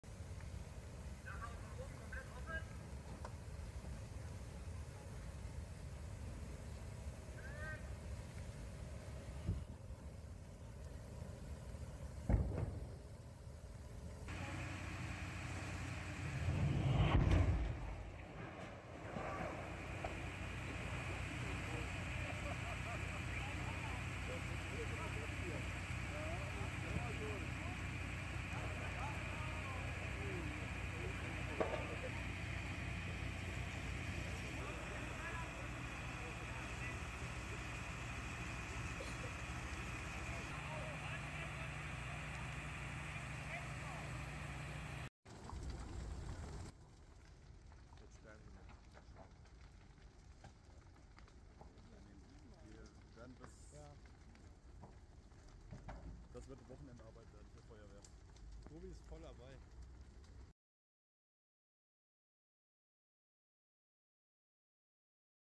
Großbrand in Hofgeismar: Gewaltiger Schaden bei Feuer in Lagerhalle
In Hofgeismar im Kreis Kassel hat es bei der Firma Siebenhaar gebrannt. Die Rauchwolke war über dem Ort deutlich zu sehen. Es entstand ein Schaden in zweistelliger Millionenhöhe.

Hast du Interesse an Videos aus Nordhessen und Südniedersachsen? Hier klicken und nichts mehr verpassen (kostenlos):